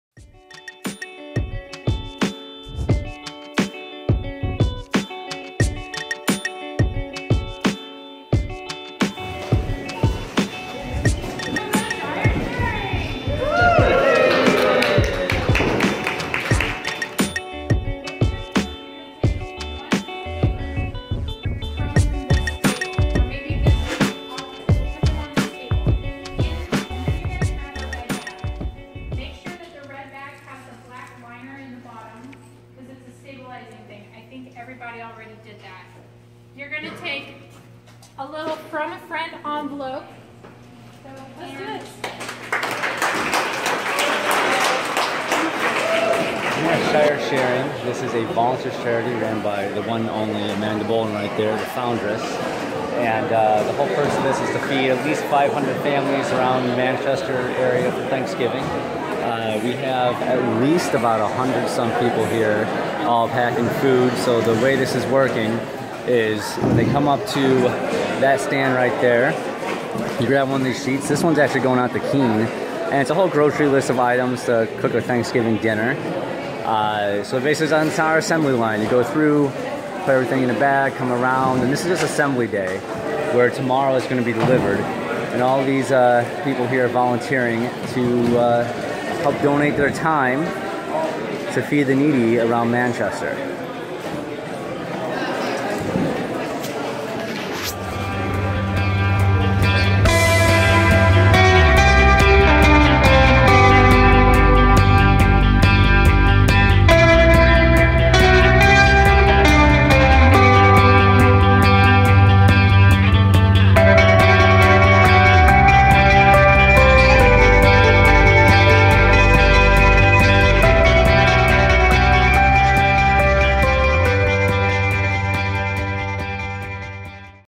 Shire Sharing 2015
Shire Sharing is a voluntaryist inspired charity organized by early mover for the Free State Project and NH State House Representative Amanda Bouldin.  On November 21, 2015 volunteers converged on a warehouse in Manchester to support the charity.  The goal of the charity is to feed 500 families in need in New Hampshire during Thanksgiving.  To learn more about the charity, go

for the full show archive